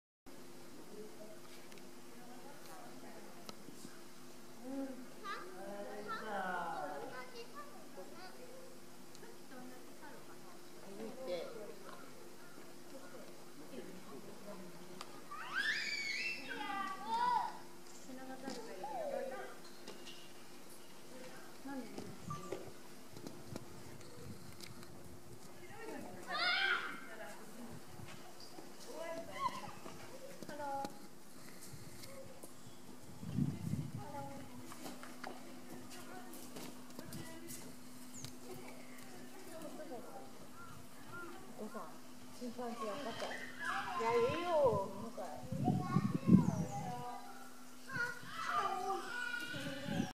お父さんちょっと疲れた
釧路市動物園のチンパンジーのゆみのすけ。
この日は天気がいいので、外で過ごしていました。
ゆづると遊んで、追いかけっこ・・・してたら疲れて休憩です。